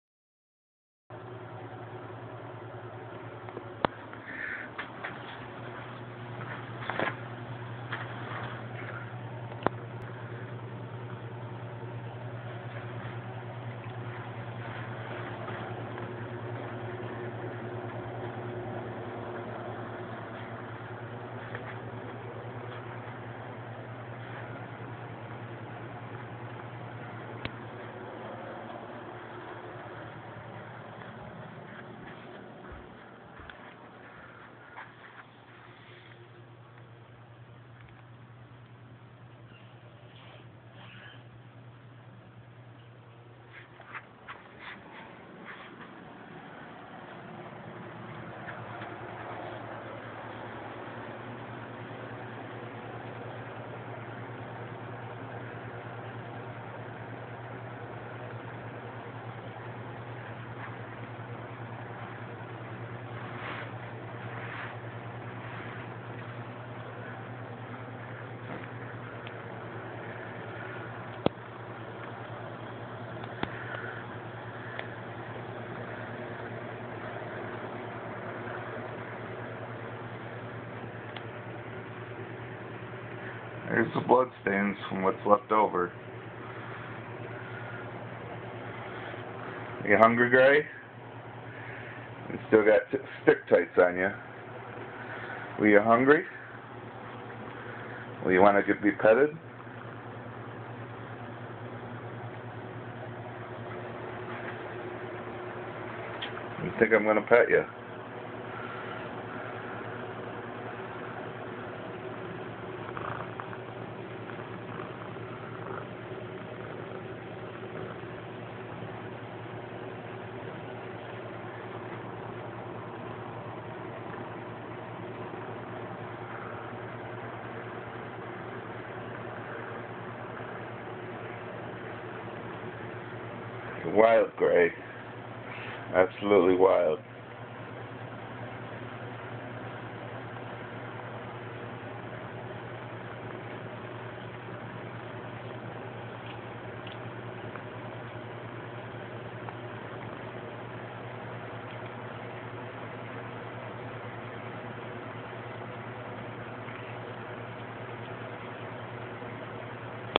Cat covered in stick tights eats mouse in bed II.
Leftovers of Missy
Grey's midnight snack.